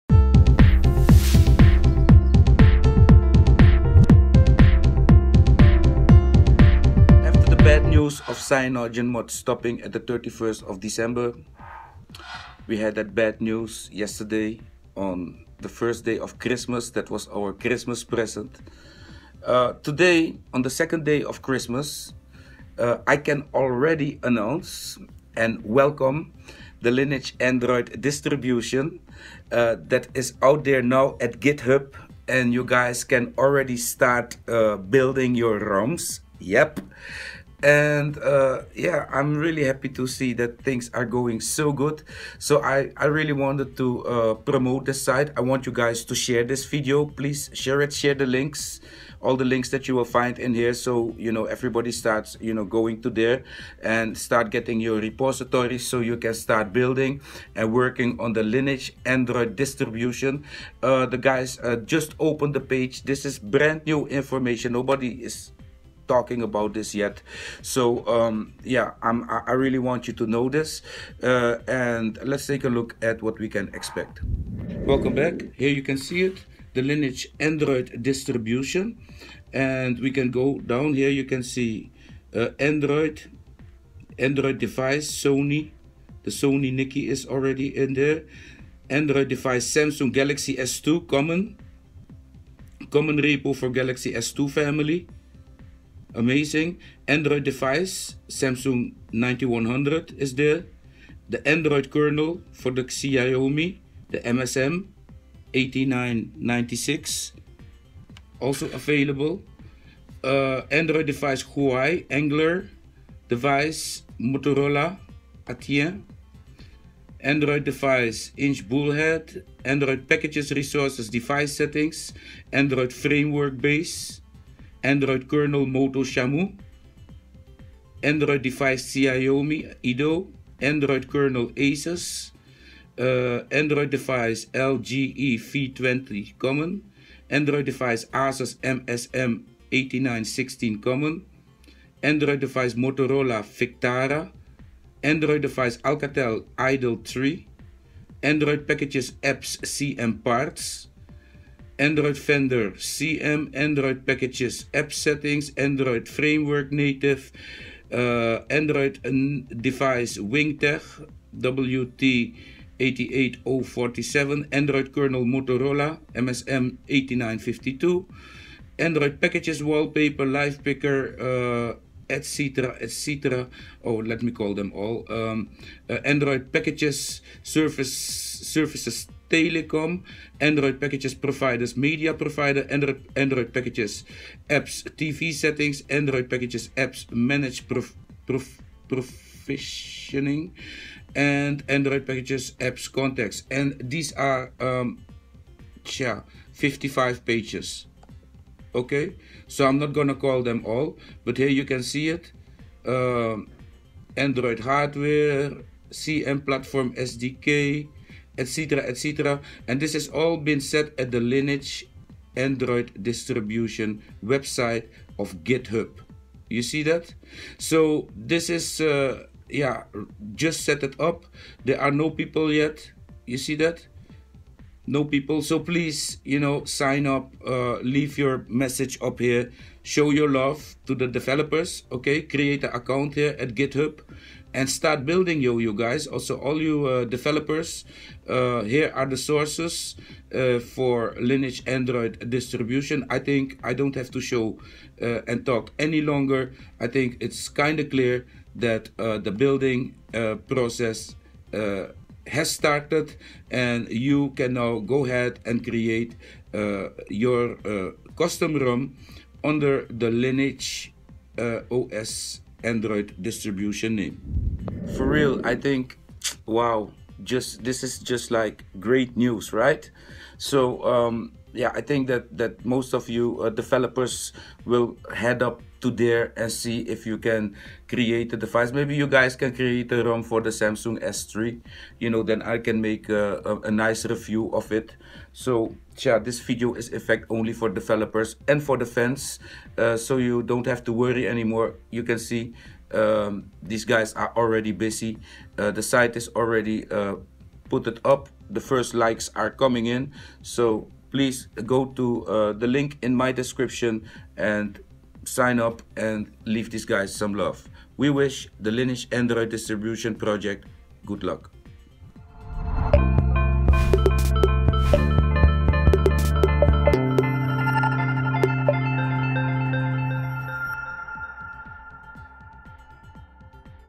LINEAGE OS ANDROID DISTRIBUTION READY FOR DOWNLOAD : THE BUILDING HAS BEGUN ....
Yes you read it very good ..Lineage Android Distribution including repo's are already at Github for download and all you developers can start building tonight already and...
have your first build ready in the morning.

Challenge who will be the first !!!
To Make one for the Samsung S3 so I will review it.

With Puck Darlington